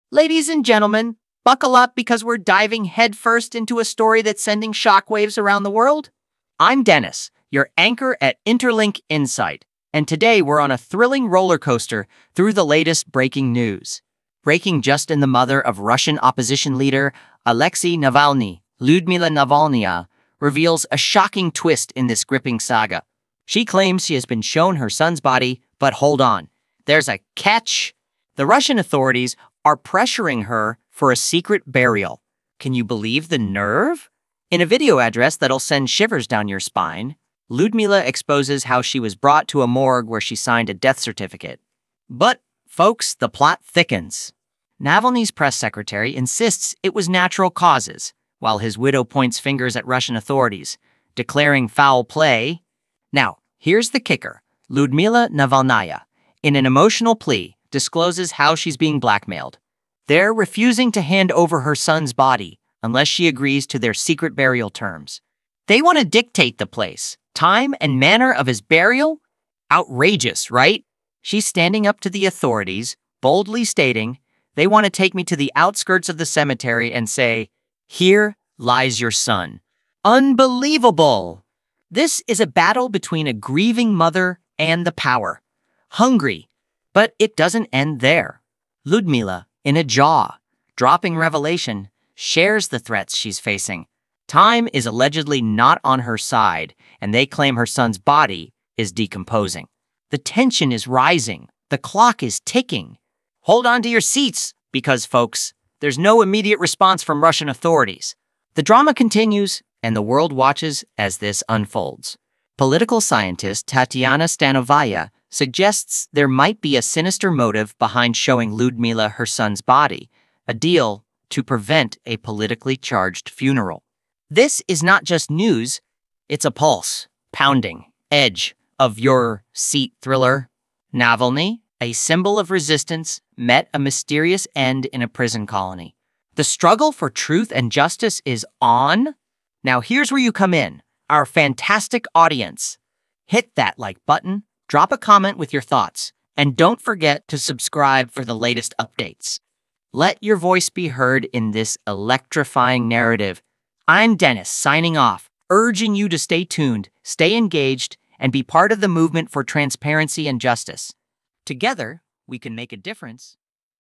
Shocking Revelation Navalny's Mother Exposes Secret Burial Plot!
Alexei Navalny, Lyudmila Navalnaya, reveals a shocking twist in this gripping saga. She claims she has been shown her son's body, but hold on, there's a catch! The Russian authorities are pressuring her for a "secret" burial. Can you believe the nerve?
In a video address that'll send shivers down your spine, Lyudmila exposes how she was brought to a morgue where she signed a death certificate. But, folks, the plot thickens! Navalny's press secretary insists it was natural causes, while his widow points fingers at Russian authorities, declaring foul play!
Now, here's the kicker – Lyudmila Navalnaya, in an emotional plea, discloses how she's being "blackmailed." They're refusing to hand over her son's body unless she agrees to their secret burial terms. They want to dictate the place, time, and manner of his burial! Outrageous, right?
She's standing up to the authorities, boldly stating, "They want to take me to the outskirts of the cemetery and say: 'Here lies your son.'" Unbelievable! This is a battle between a grieving mother and the power-hungry.
But it doesn't end there. Lyudmila, in a jaw-dropping revelation, shares the threats she's facing. Time is allegedly not on her side, and they claim her son's body is decomposing. The tension is rising, the clock is ticking!
Hold on to your seats because, folks, there's no immediate response from Russian authorities. The drama continues, and the world watches as this unfolds. Political scientist Tatiana Stanovaya suggests there might be a sinister motive behind showing Lyudmila her son's body – a deal to prevent a politically charged funeral.
This is not just news; it's a pulse-pounding, edge-of-your-seat thriller! Navalny, a symbol of resistance, met a mysterious end in a prison colony. The struggle for truth and justice is on!
Now, here's where you come in, our fantastic audience! Let your voice be heard in this electrifying narrative.